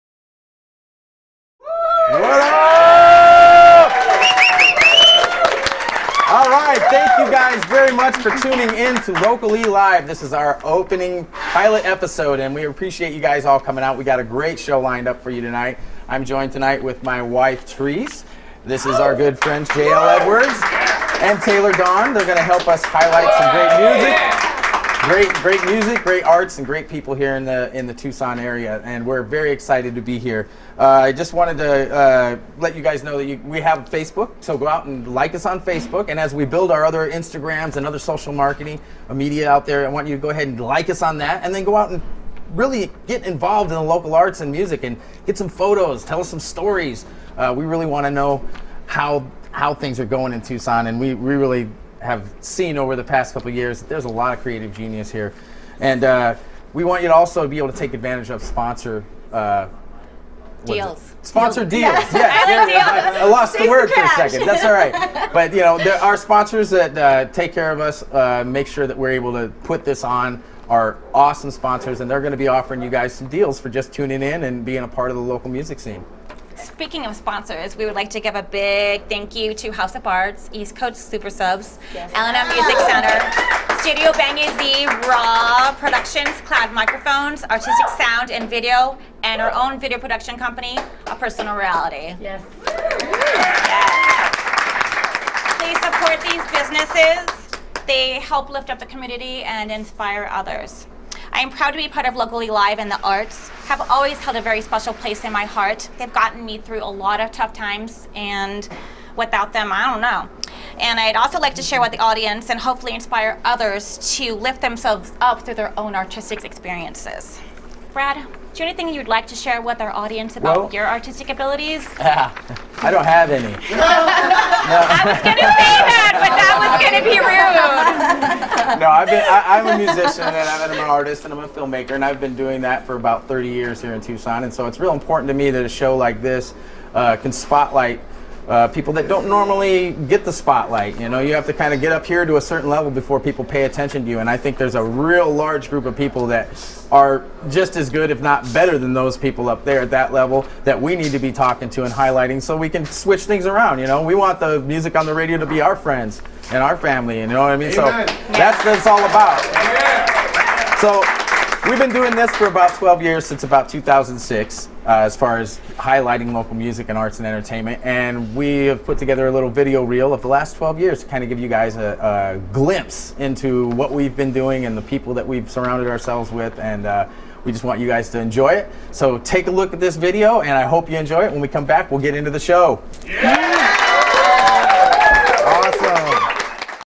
Local e Live, Seg. 1 intro
Local e Live was an online show produced by APR video. Hosted by my wife Therese, good friends JL Edwards and Taylor Dawn, and myself. We interviewed local Tucson artists, musicians, and of course our show sponsors. This was the only live audience show we did. Check out the many other interviews we did at live shows on our